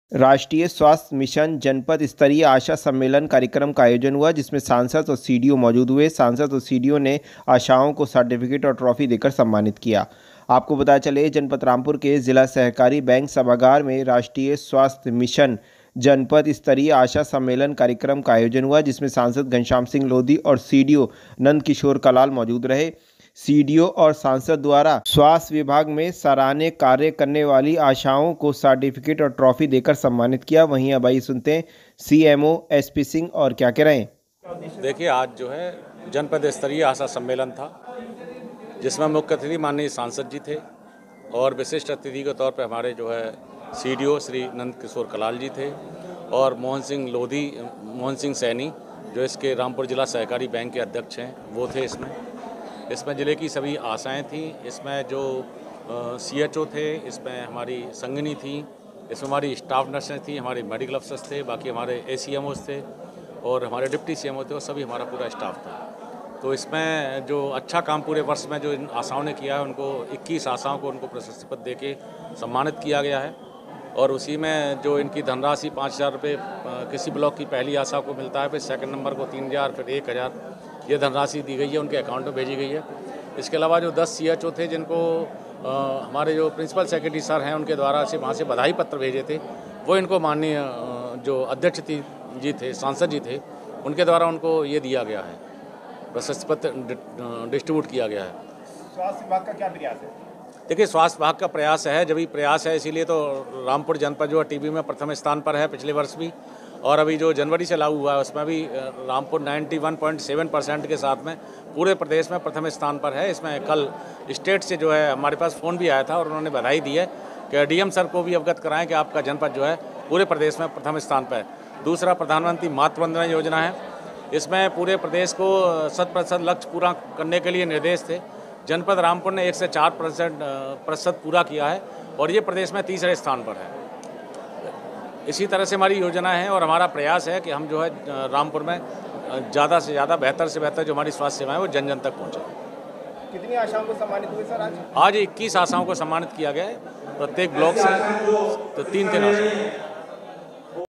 Rampur: राष्ट्रीय स्वास्थ्य मिशन के अन्तर्गत जनपद में कार्यरत आशा को सम्मानित कार्यक्रम आयोजित हुआ
Rampur: राष्ट्रीय स्वास्थ्य मिशन के अन्तर्गत जनपद में कार्यरत आशा कार्यकत्रियों को सम्मानित कार्यक्रम आयोजित हुआ